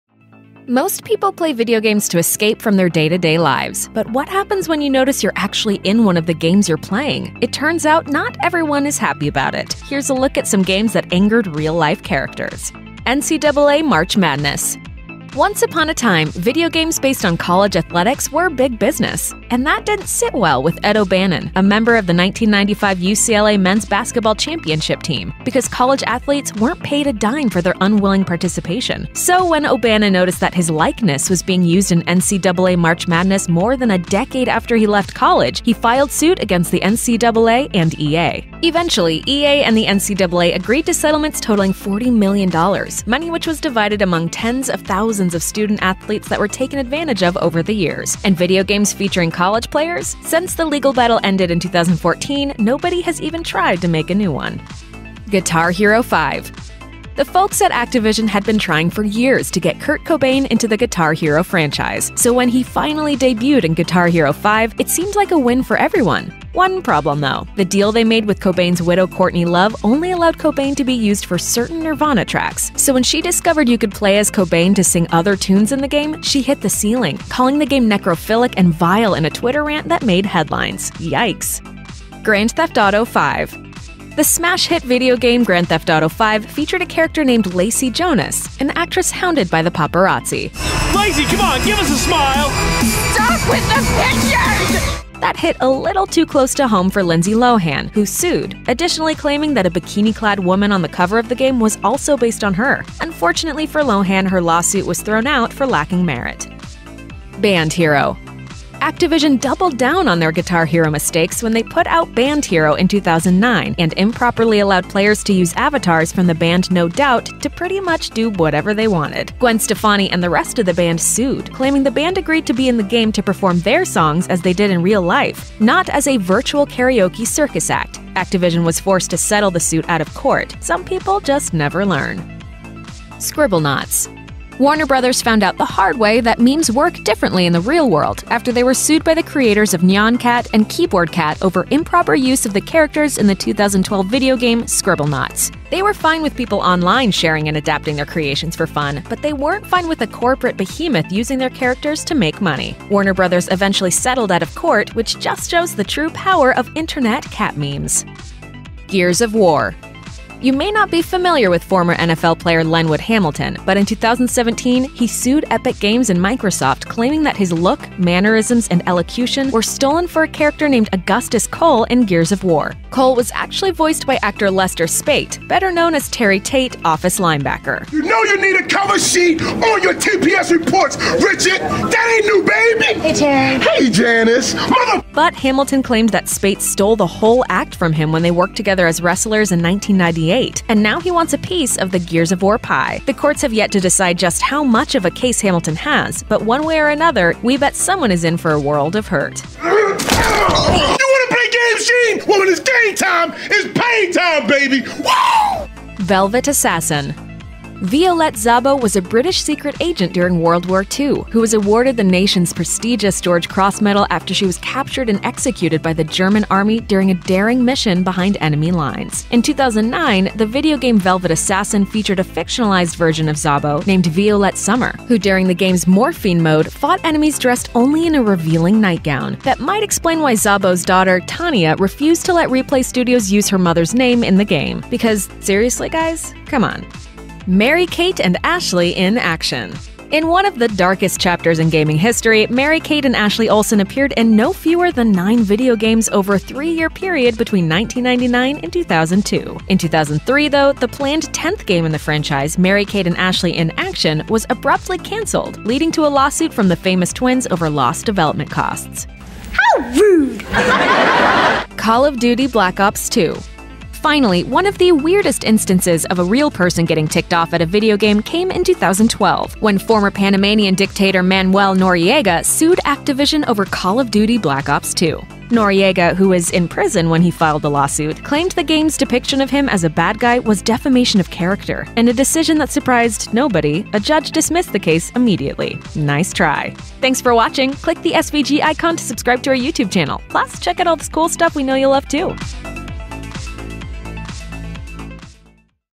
Games That Angered Real Life Characters
While many video games introduce players to unique and interesting fictional characters, others use real-life people as characters. While they have to reach an agreement with the person (or their estate) to actually use them in the game, there have been times that the companies have been accused of being dishonest in their contracts. Other times, they have created characters who look a whole lot like famous people that they claim are actually unique fictional characters. From Courtney Love’s gripe with Guitar Hero 5 to the athletes who took legal action against an NCAA game, let’s take a look at some games that angered real-life characters.

NCAA March Madness | 0:00
Guitar Hero 5 | 0:57
Grand Theft Auto V | 1:24
Band Hero | 1:48
Scribblenauts | 2:14
Gears of War | 2:40
Velvet Assassin | 3:27
Mary-Kate and Ashley in Action! | 3:59
Call of Duty: Black Ops II | 4:23